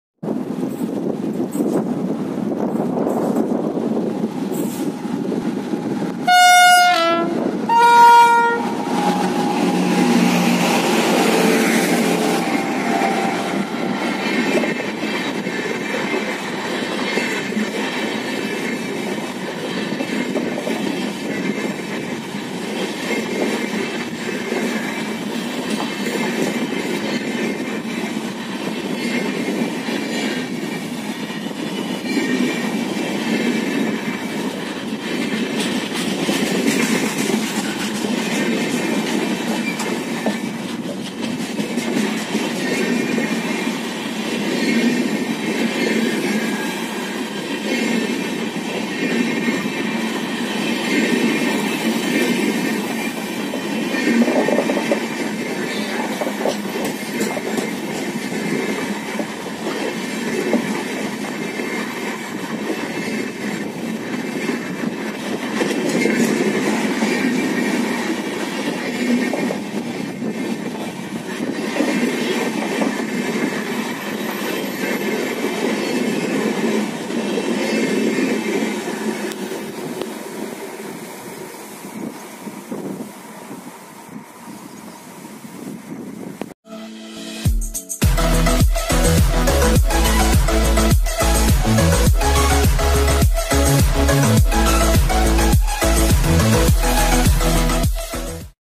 [The crawl show] Puri 🔁 Okha Dwarka express with Lalaguda WAP7 towards murtizapur
hello friends this is railfan tushar from akola presenting one more video of lhb train
train  name - okha Dwarka express
locomotive - lalaguda WAP7
device - redmi note 5 pro
edit - rf tushar
shot by - rf tushar